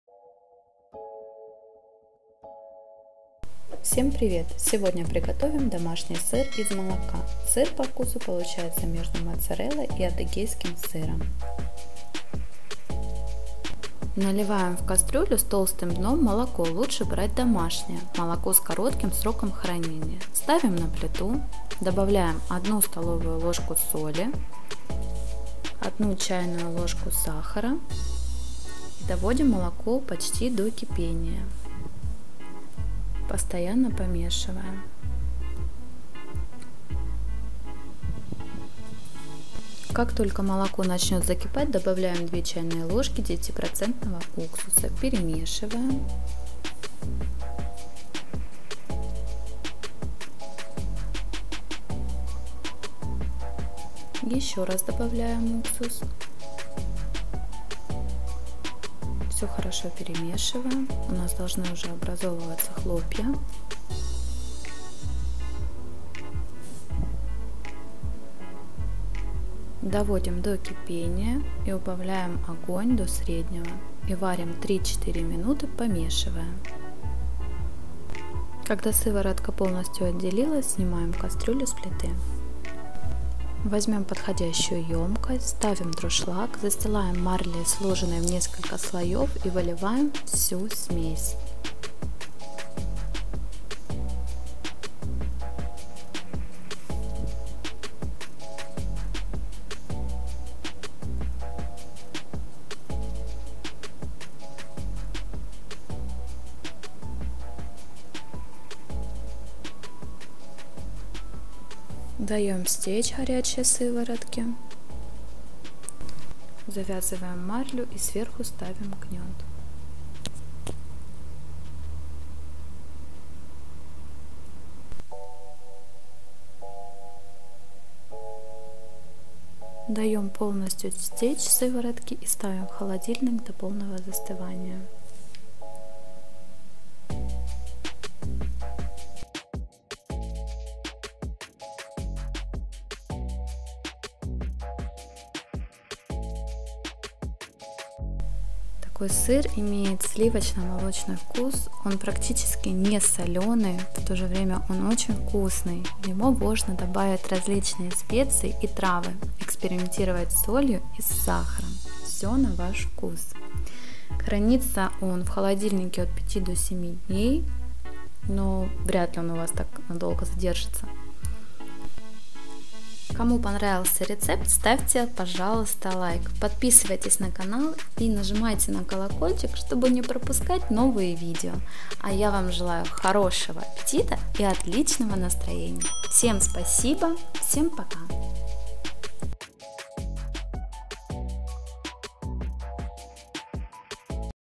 Домашний СЫР 🧀 из молока 🥛 за 15 МИНУТ. Супер БЫСТРЫЙ РЕЦЕПТ 🕘
Сыр в домашних условиях из молока намного вкуснее и полезнее.
Приготовим простой рецепт сыра который даже новичок справится с ним.
Нам понадобиться
3 л молока ;
1 ст.л соли ;
1 ч.л сахара ;
6 ч.л уксуса 9% ;

Сыр получается весом 400г.